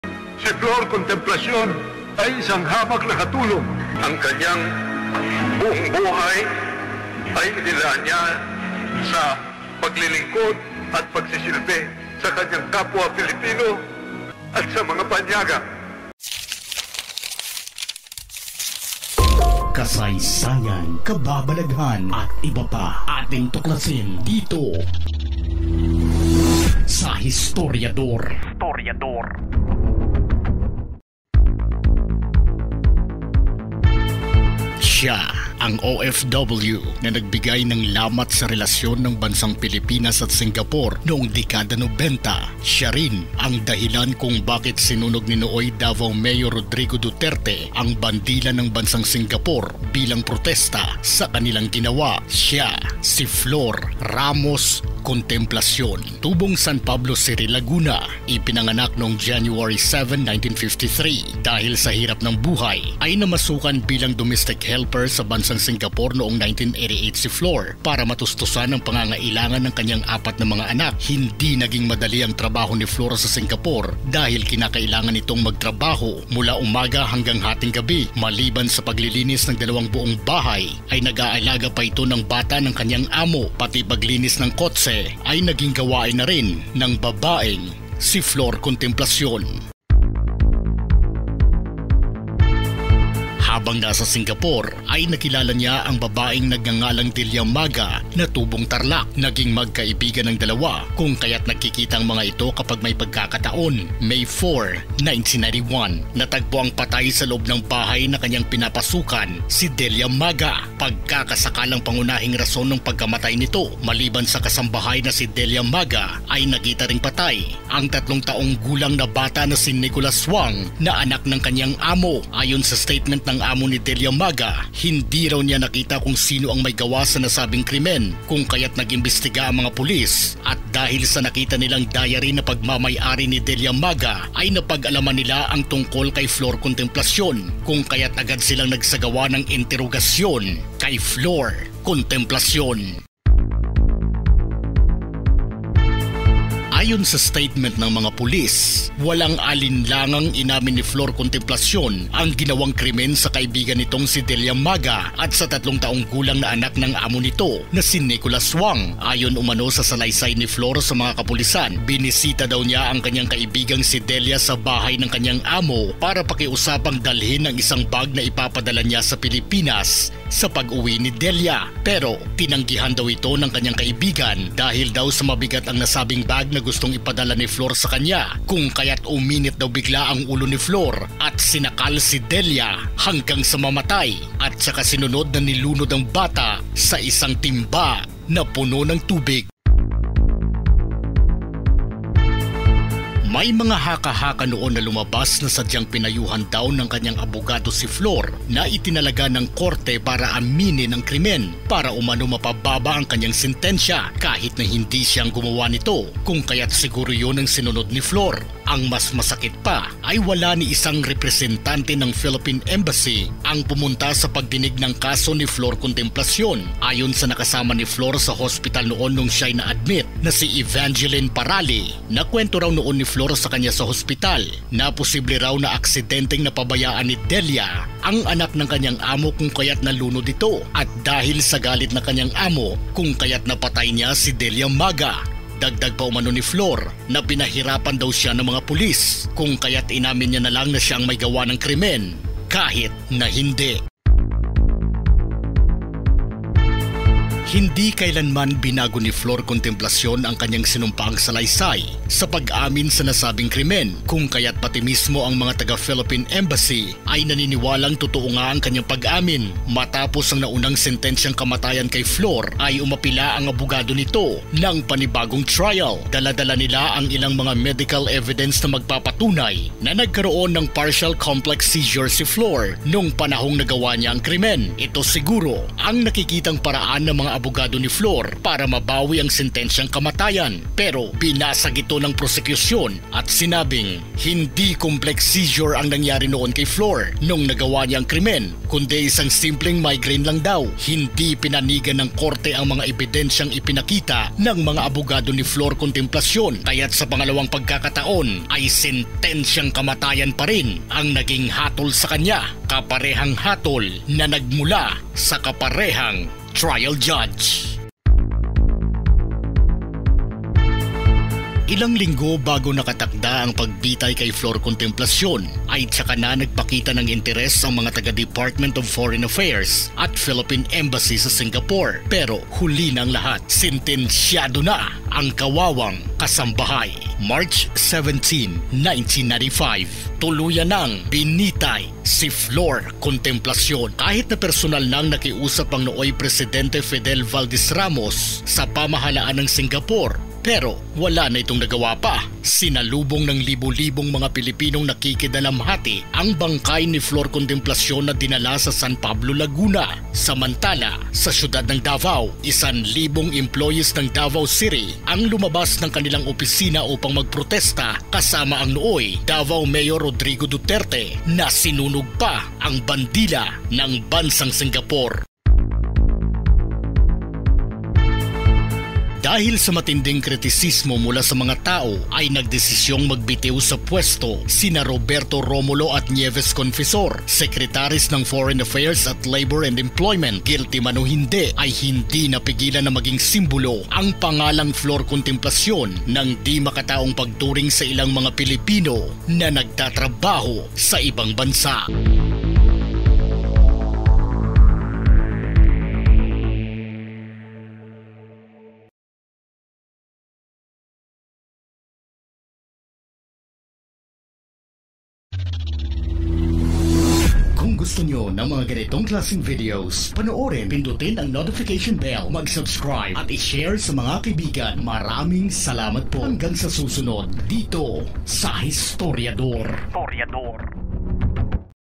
FLOR CONTEMPLACION
Historiador Filipino#Flor Contemplacion - Alamin ang istorya kung bakit hinayaang mabitay si Flor Contemplacion!